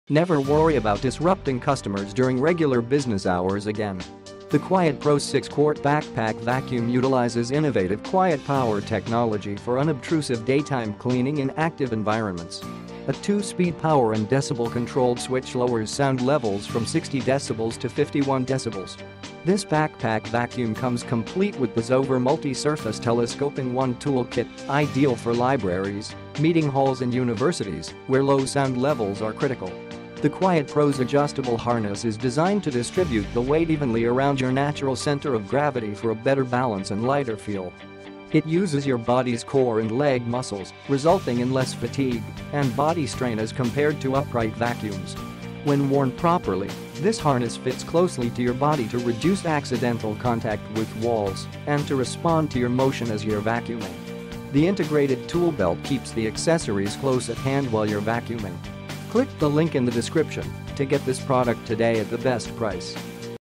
ProTeam 107146 Commercial Backpack Vacuum QuietPro BP Vacuum Backpack HEPA Media Filtration Xover
☑️ Review

QUIETPOWER TECHNOLOGY - Never worry about disturbing guests and residents with 2 power modes that deliver high performance where low sound levels are critical

BREATHE BETTER AIR - Improve Indoor Air Quality (IAQ) for your guests with Four Level Advanced Filtration and HEPA media filter; Carpet and Rug Institute (CRI) Certified Bronze

COVER MORE GROUND - The extra-long 50-foot power cord allows cleaning of large areas without having to unplug to easily maneuver around the furniture

IMPROVE CLEANLINESS - Backpack vacuum comes loaded with accessories perfect for cleaning hard floors, carpets, stairs, upholstery and more

Ideal for daytime cleaning environments such as senior living facilities, daycares, apartment buildings, health clubs, and retail spaces

This channel called Nikki Morissette is a participant in the Amazon Associates Program, which means we earn from qualifying purchases